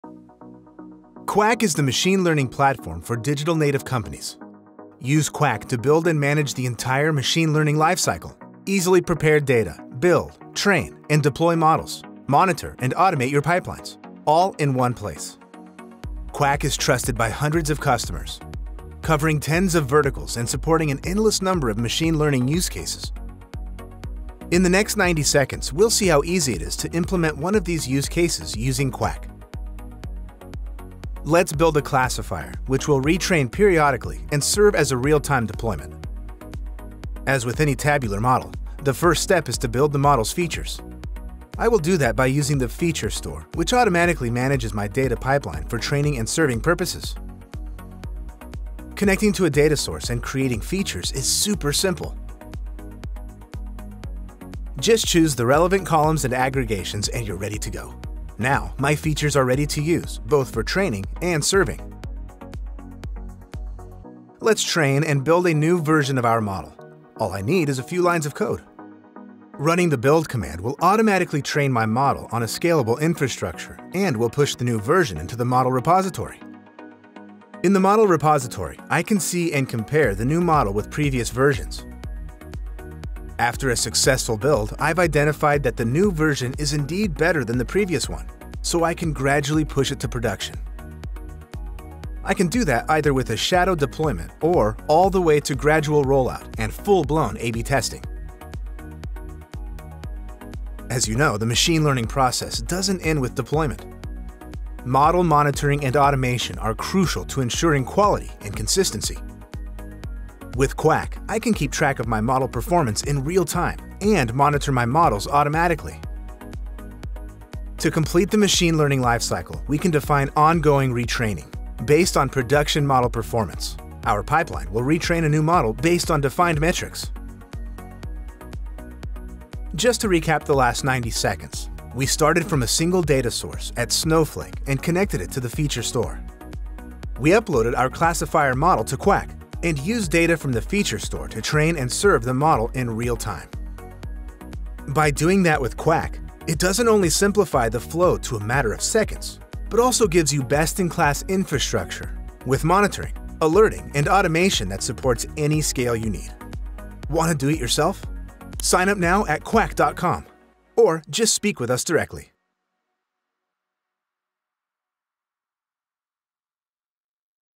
Qwak - The fully managed end to end ML platform
A fully managed platform that unifies ML engineering and data operations - providing an agile infrastructure that enables the continuous productionization of ML models at scale.